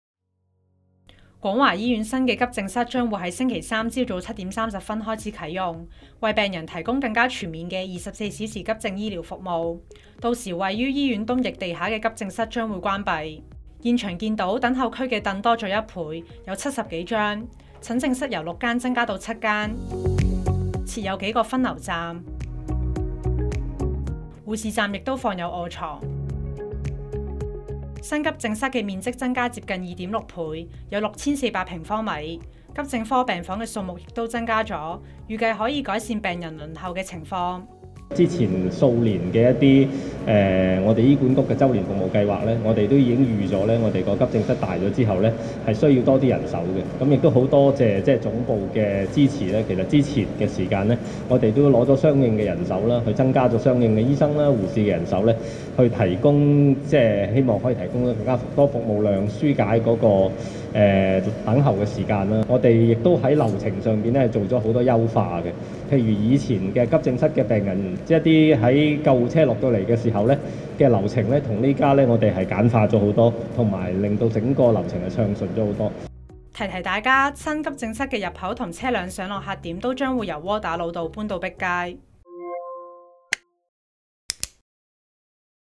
廣華醫院新急症室周三啟用 面積增近2.6倍 料可改善輪候情況｜01新聞｜廣華醫院｜急症室｜醫療｜碧街｜鄧錦成
廣華醫院醫院重建計劃第一期新大樓已經竣工，新急症室亦將於周三（31日）早上7時30分起啟用，為病人提供更全面的 24小時急症醫療服務，入口將由現時的窩打老道遷至碧街。廣華醫院醫院行政總監鄧錦成醫生指新急症室的面積較舊急症室增加近2.6倍，達6,400平方米，並增設急症科病房、感染控制隔離區域等，診症室的數目亦有所增加，再配合新增的急症科病房，預期有助改善病人輪候的情況。